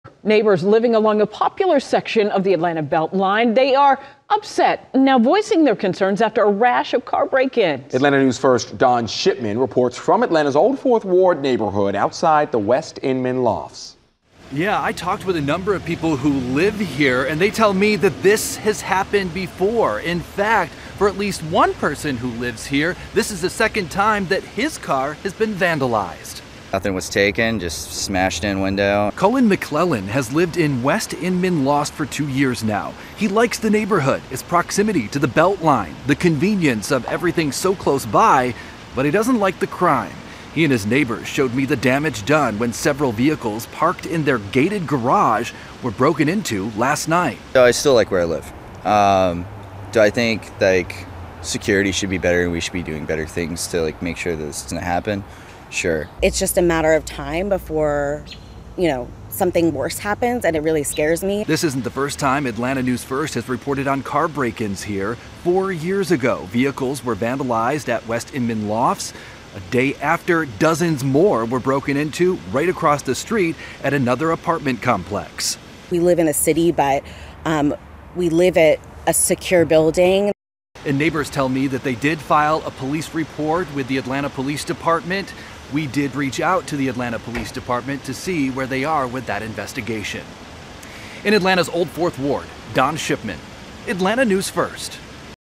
Old Fourth Ward residents concerned after another rash of car break-ins
“Nothing was taken. Just a smashed-in window.”